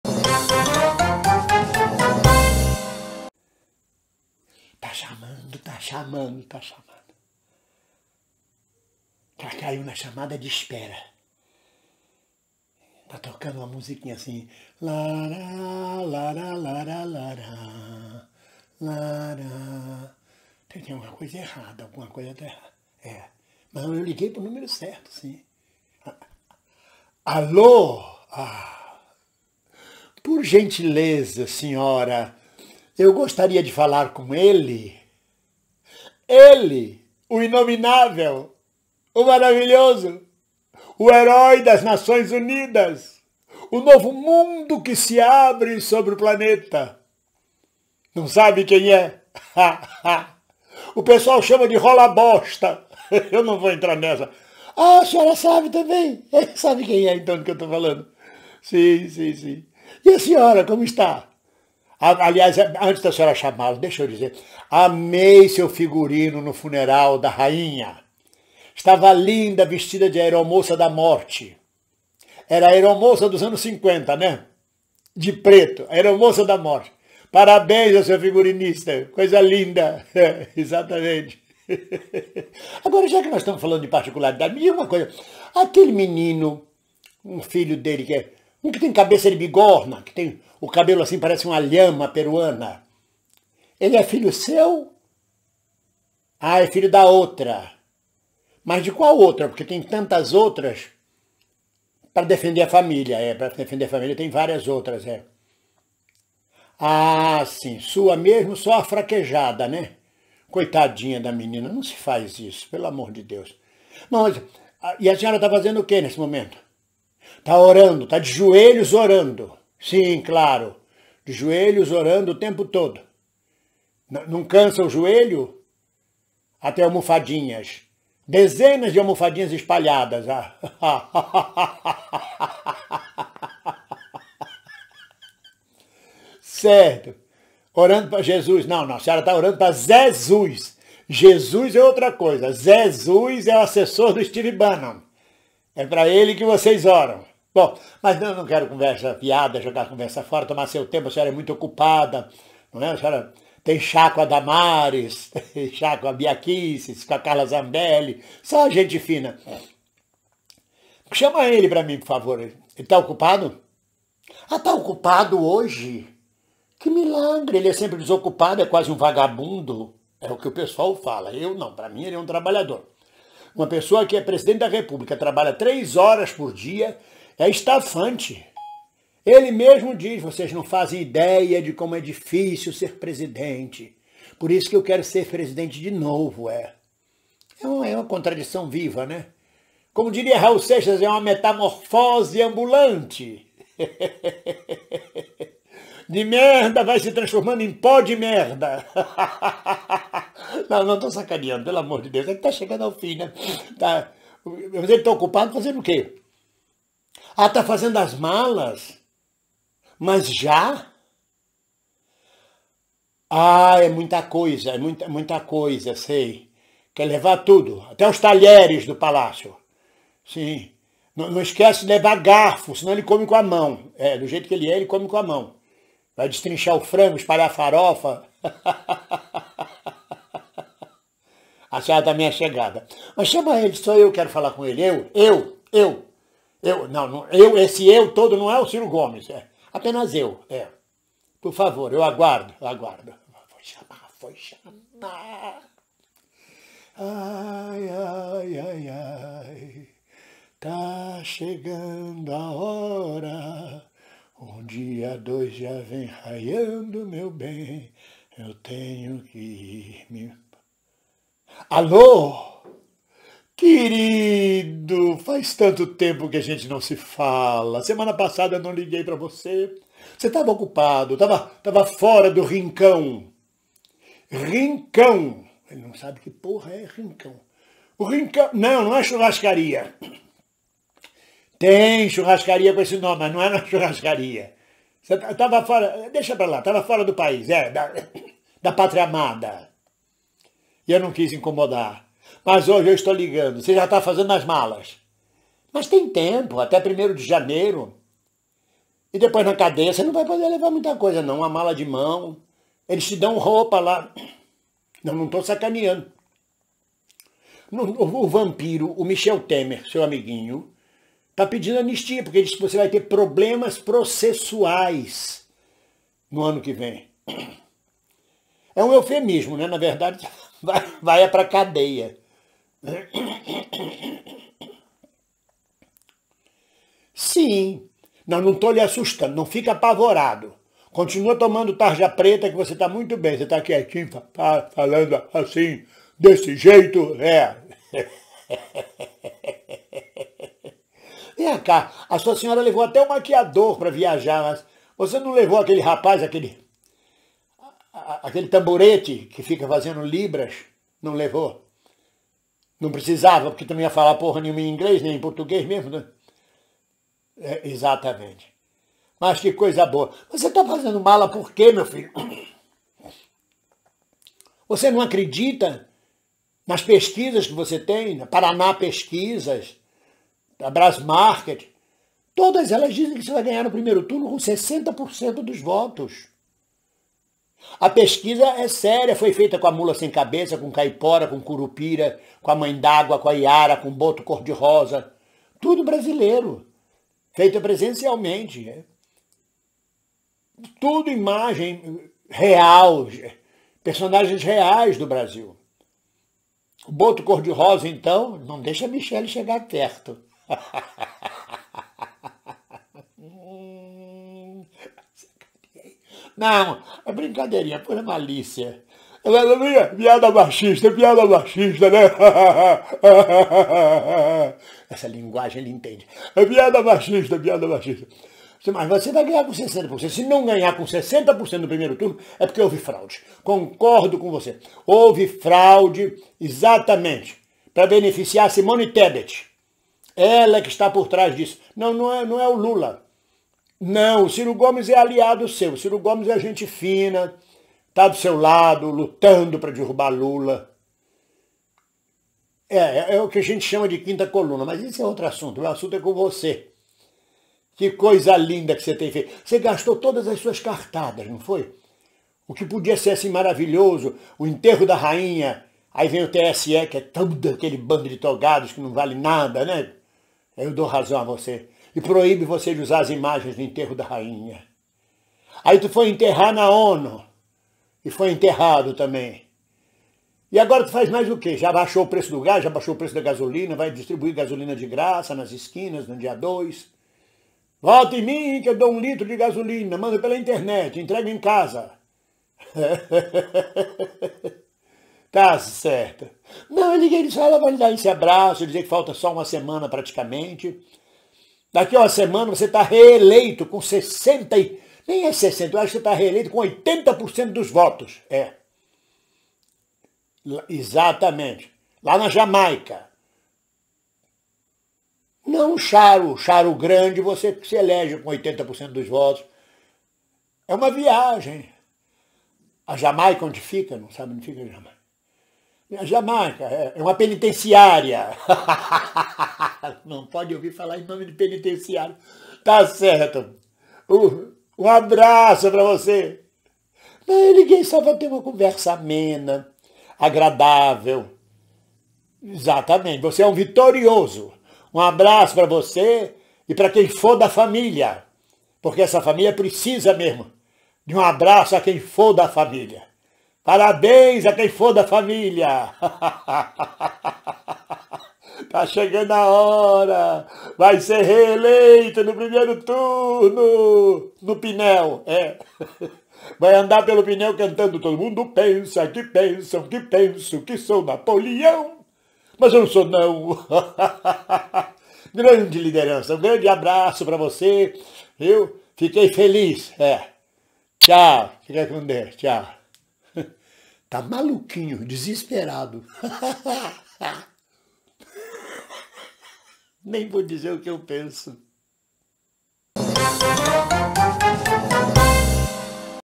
TELEFONEMA PRO INOMINÁVEL. CADA DIA MAIS DESESPERADO, JÁ ESTÁ FAZENDO AS MALAS.
Humor, sátira política.
Seja membro deste canal e ganhe benefícios:
Obs.: Os comentários dos espectadores são de responsabilidade dos próprios e podem nãocorresponder à minha opinião.